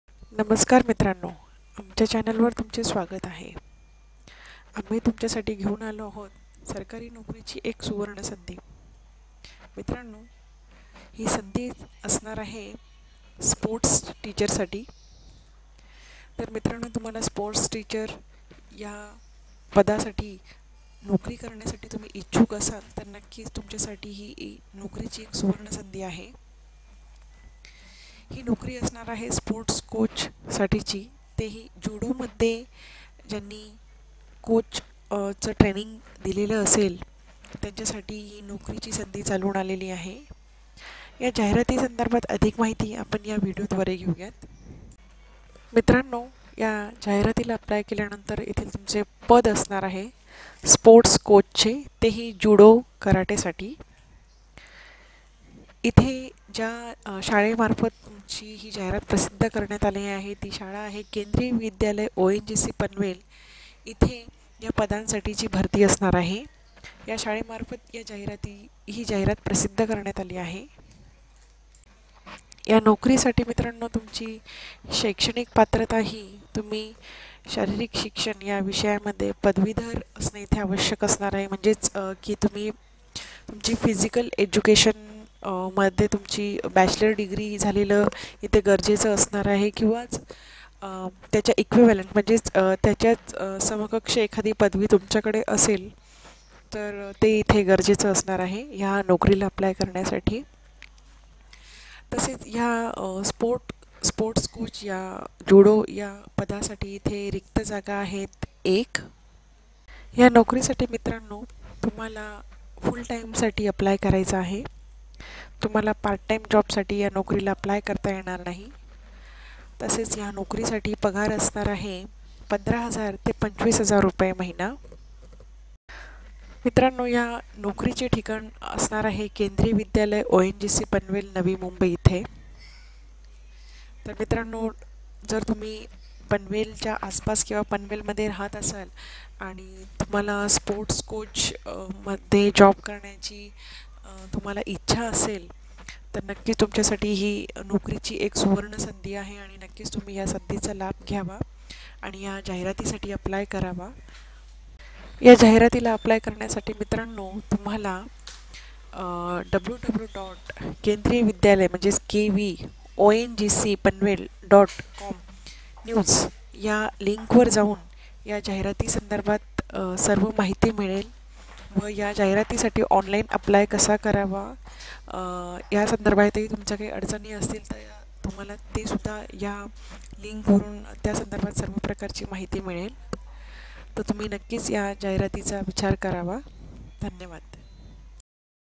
केंद्रीय विद्यालय ओएनजीसी पनवेल स्पोर्ट्स कोच पदांसाठी भरती स्पोर्ट्स कोच (ज्युडो) पदासाठी भरती
स्पोर्ट्स कोच - ज्युडो | केंद्रीय विद्यालय ओएनजीसी पनवेल

पोस्टः स्पोर्ट्स कोच - ज्युडो

शाळा: केंद्रीय विद्यालय ओएनजीसी पनवेल

रिक्त पदांची संख्या: ०१

शिक्षण: शारीरिक शिक्षण या विषयात पदवीधर किंवा समकक्ष पदवी.

नोकरीची वेळ: पूर्ण वेळ

पगार: रु. 15000- रु. 25000 महिना

स्थळ: केंद्रीय विद्यालय ओंगसी, पनवेल नवी मुंबई